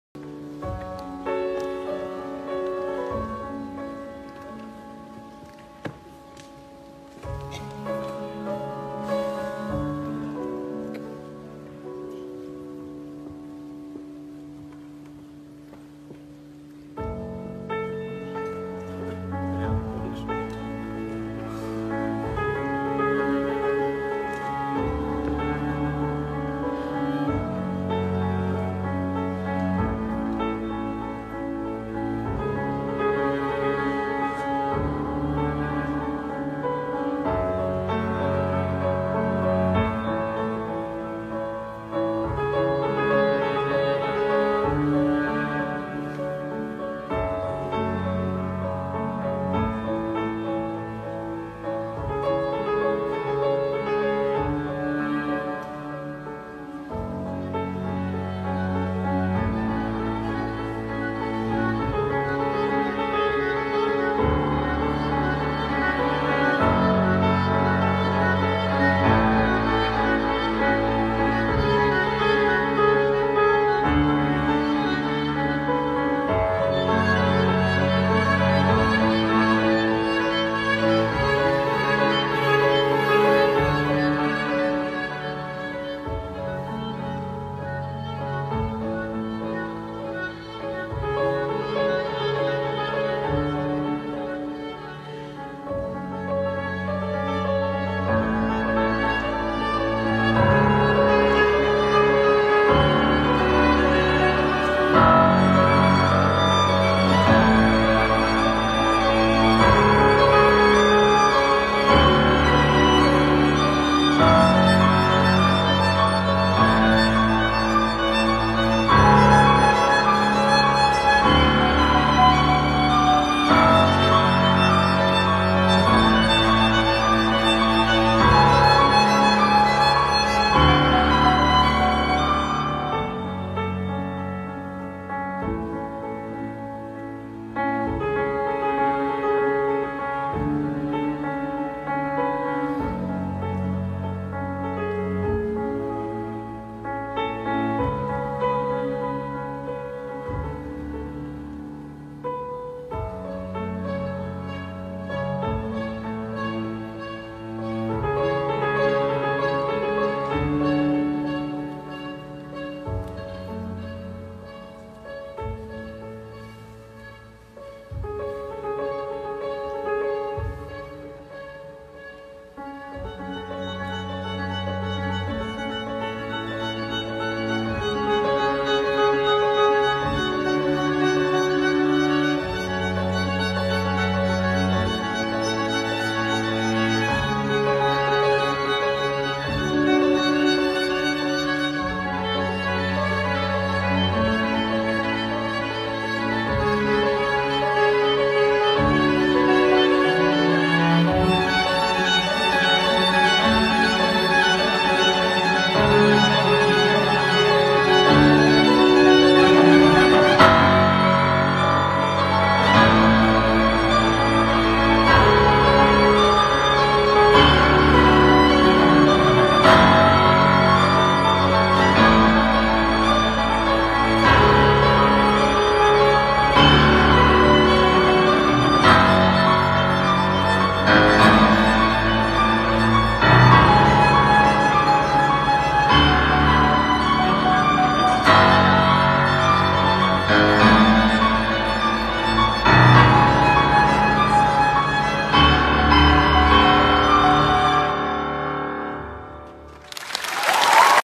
Ludovico Einaudi concert 14.11.2021
Concert of talented pianist Ludovico Einaudi in Kyiv, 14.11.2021
I like this great person very much! Thank you, @ludovicoeinaudi !